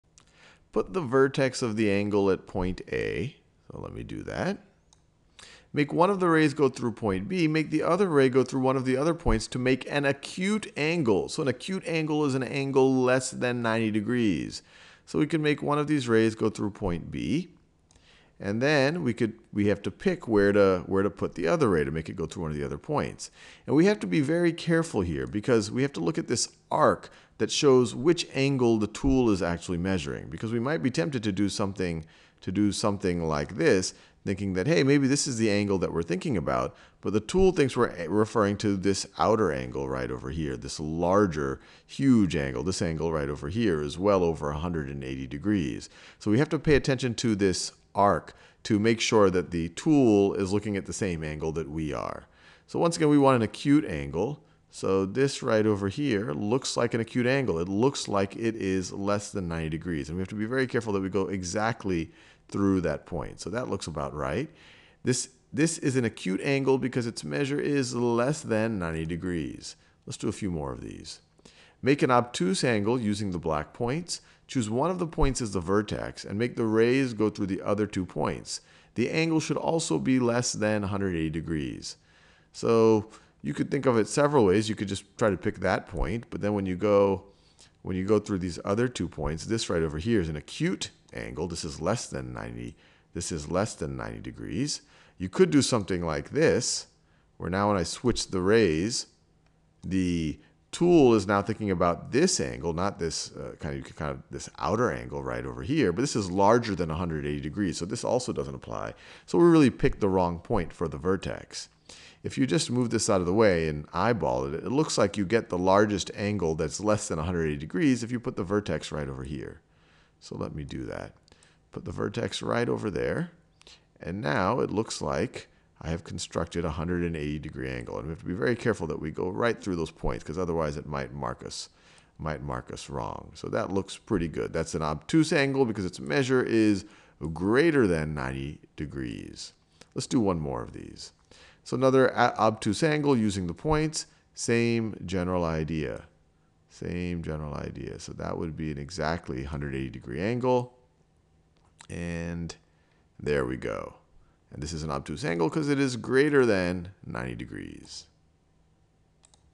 Drawing acute, right and obtuse angles | 4th grade | Khan Academy
These 3 types of angles will often come up in your geometry studies. Let's make sure you know how to draw an acute, obtuse, and right angle.